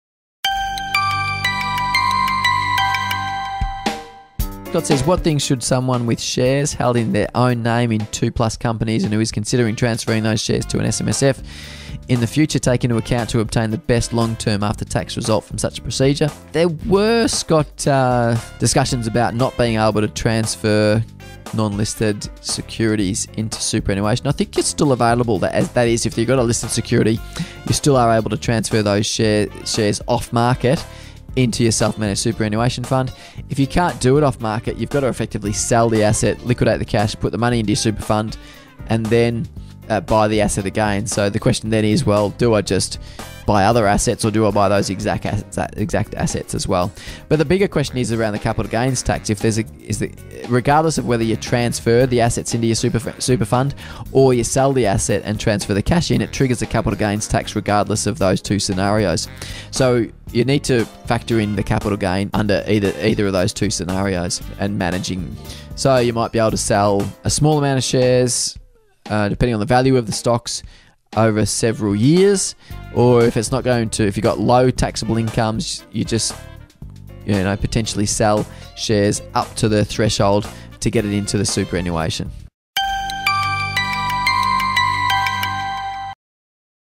46 Scott asks "Should I transfer my shares into a self-manage super fund?"
Gavin Martin, Financial Adviser and the Managing Director of Cornerstone Wealth answers a participant’s question.